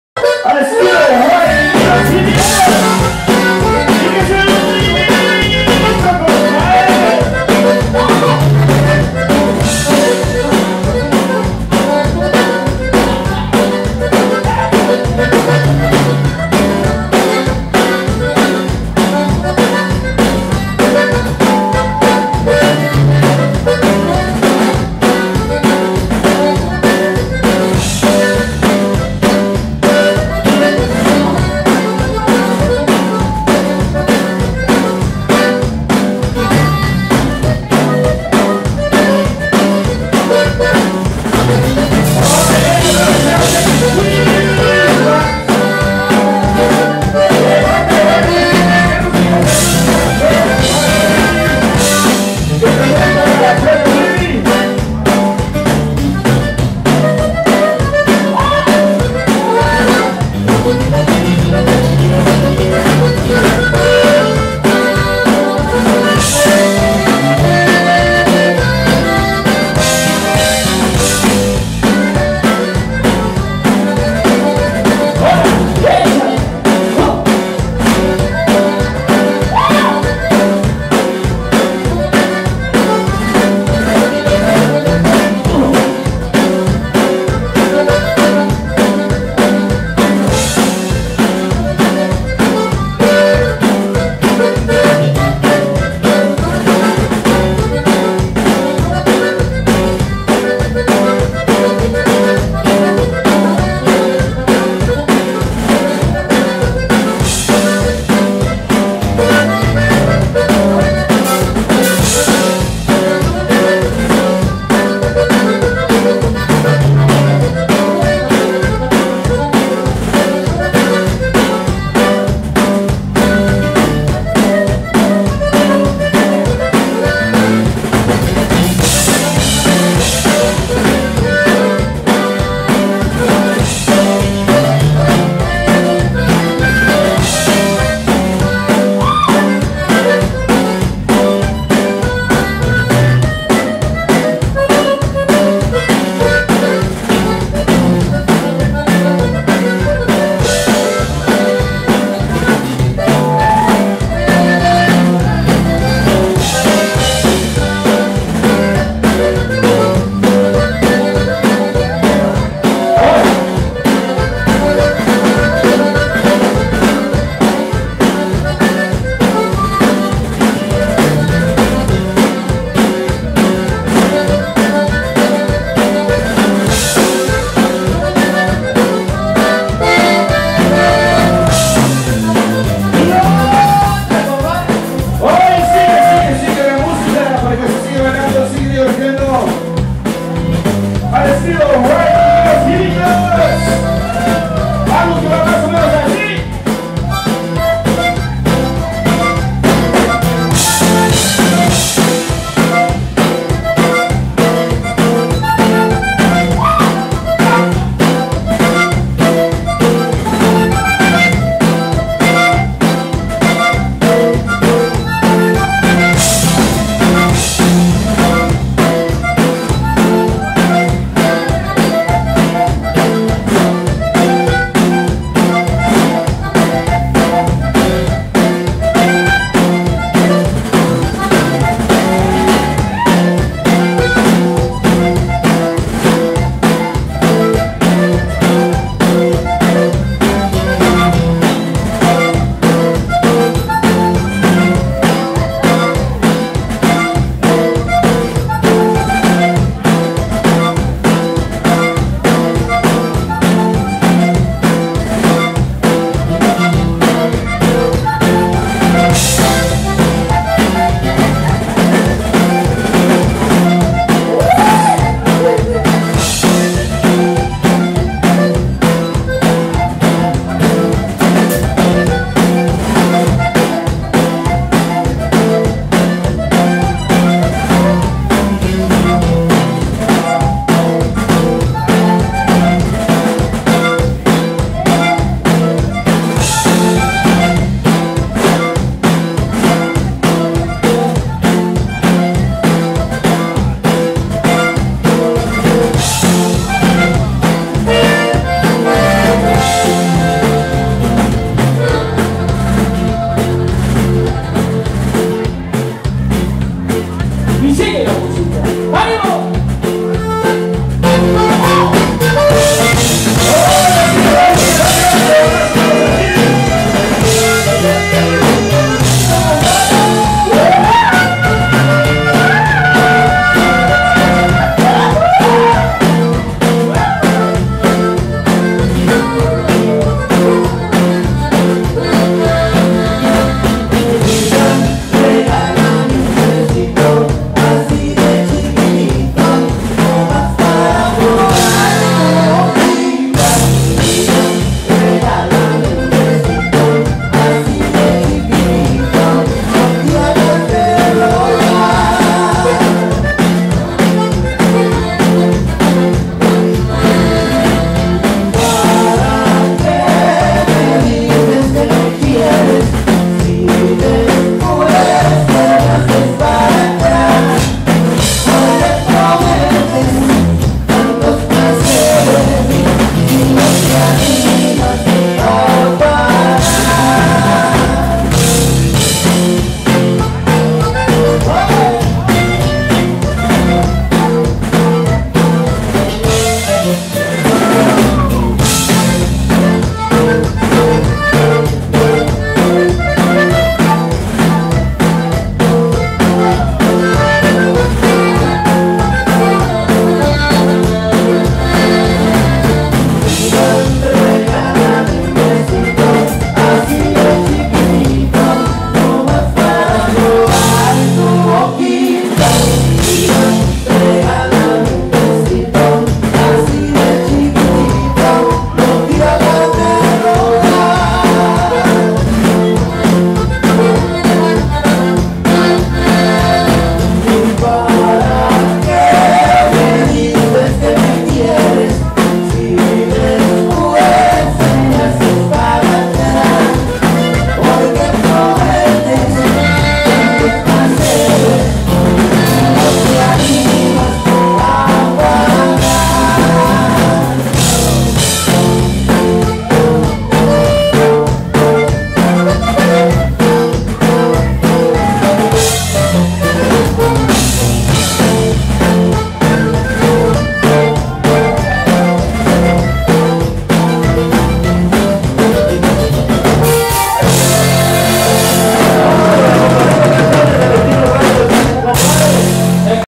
Riley y Los Gilitos 3
Riley y Los Gilitos at Gas Light Club in McAllen, TX on 11/06/15.